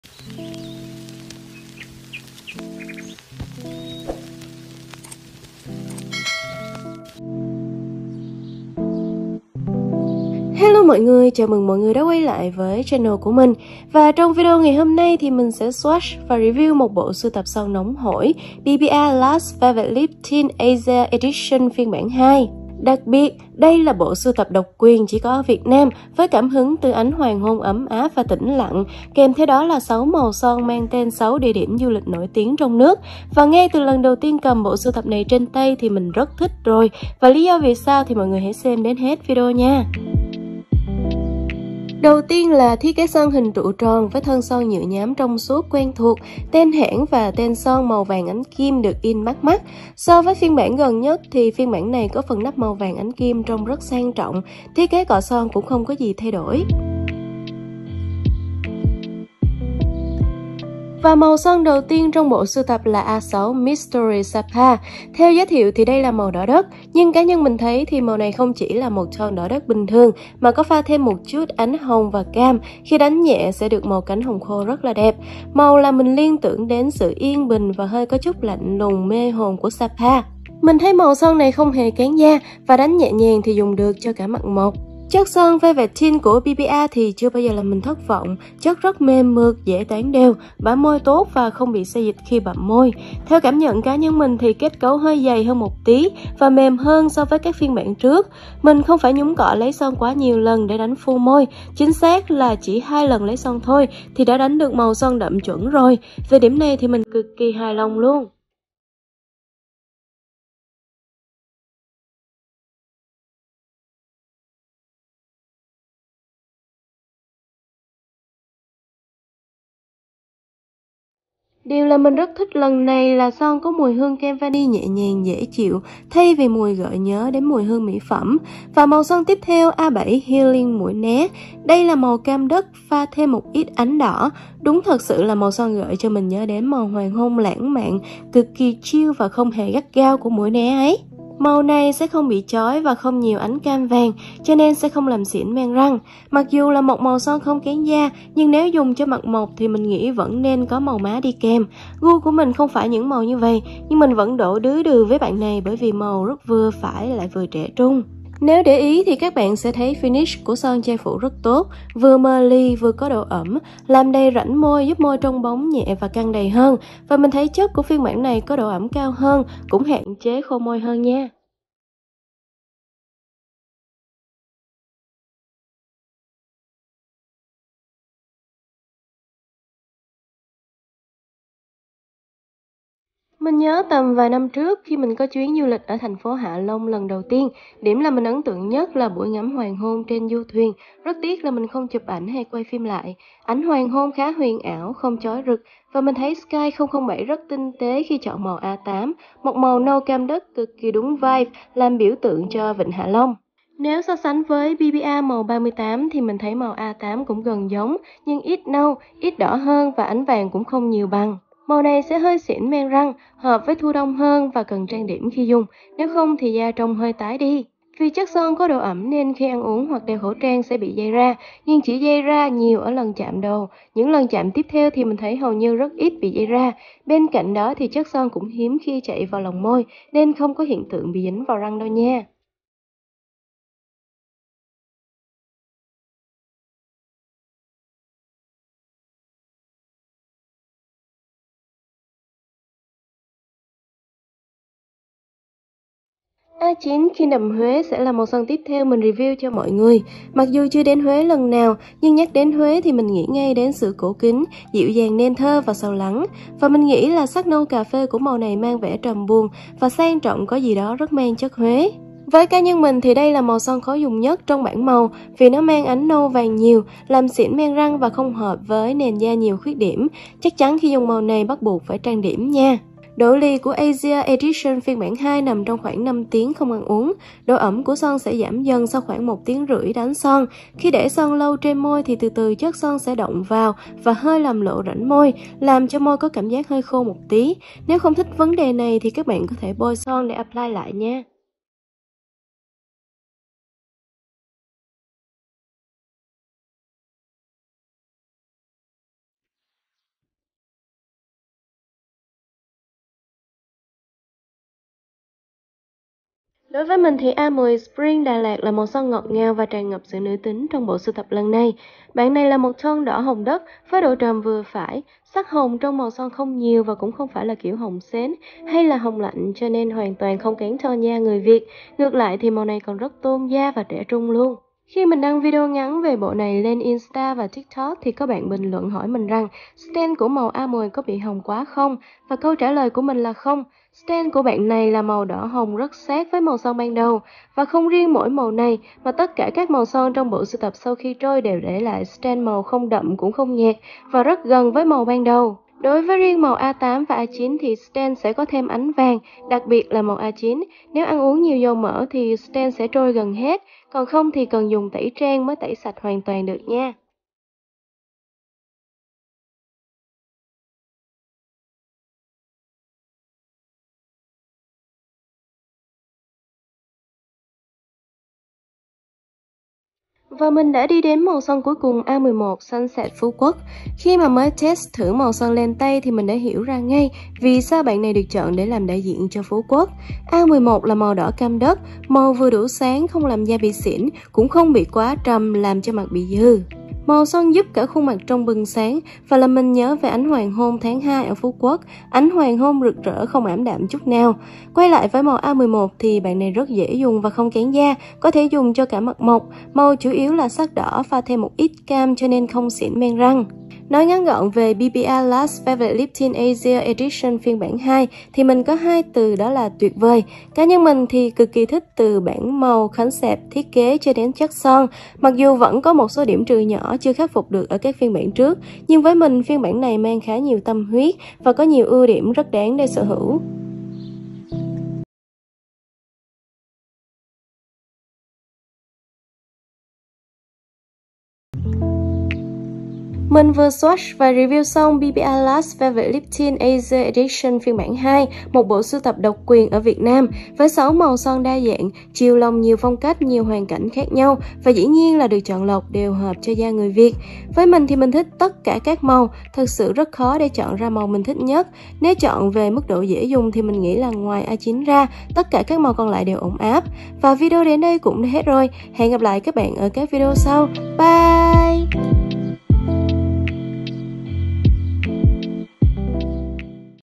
Bộ sưu tập son độc quyền ở Việt Nam | BBIA Last Velvet Lip Tint Asia Edition Version 2 | Fung Chen
Bbia Last Velvet Lip Tint Asia Edition Version 2

Ánh sáng video: ánh sáng tự nhiên không đèn (mình quay ở cửa sổ)
Undertone: Neutral
Skintone: Trung bình sáng

Mình quay video bằng iphone 12 pro max